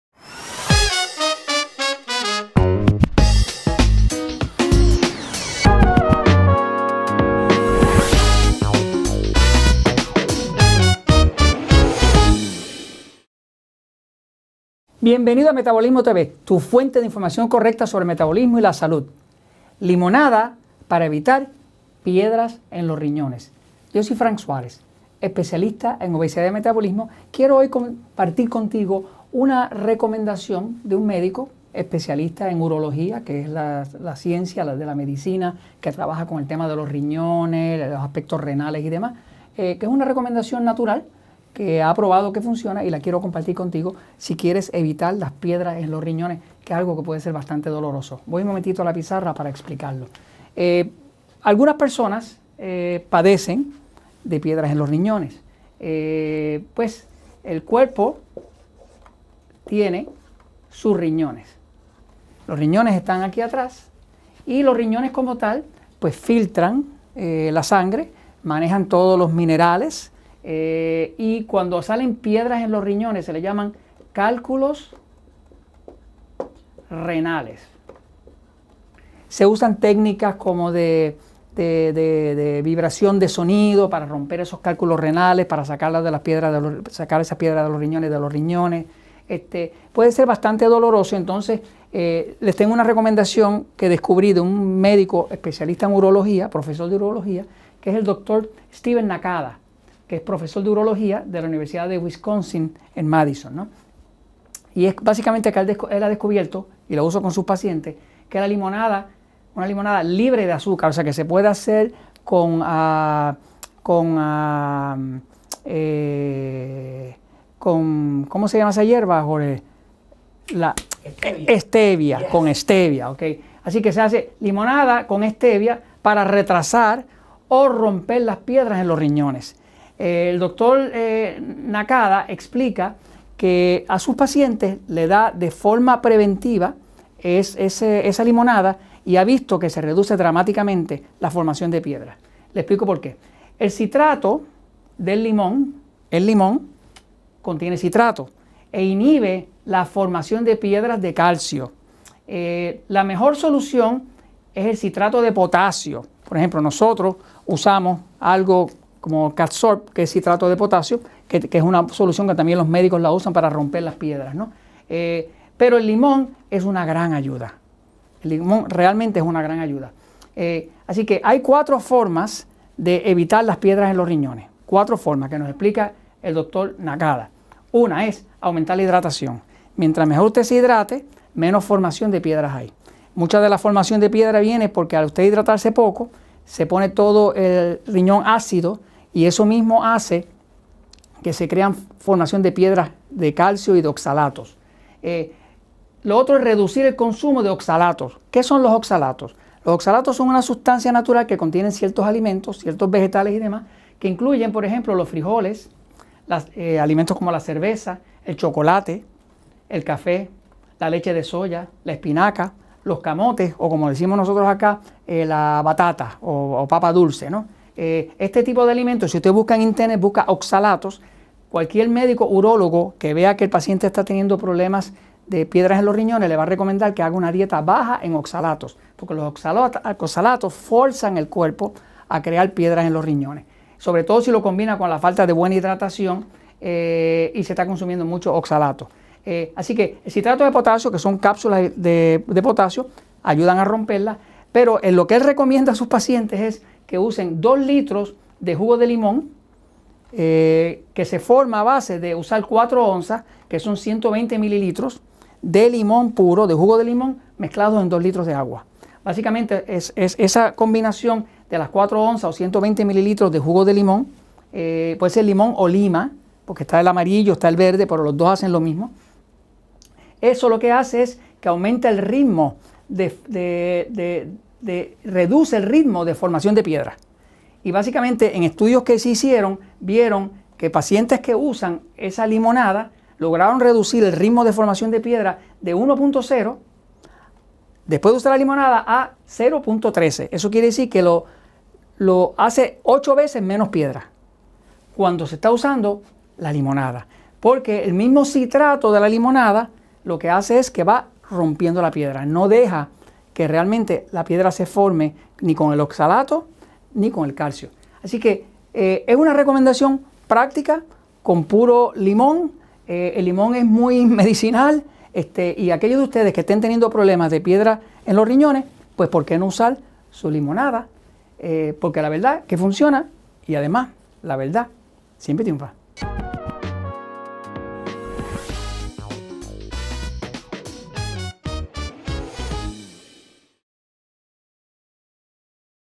Episodio #1906 Limonada Para Evitar Piedras En Los Riñones
Para obtener la ayuda de un Consultor Certificado en Metabolismo hable al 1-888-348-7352 o visítenos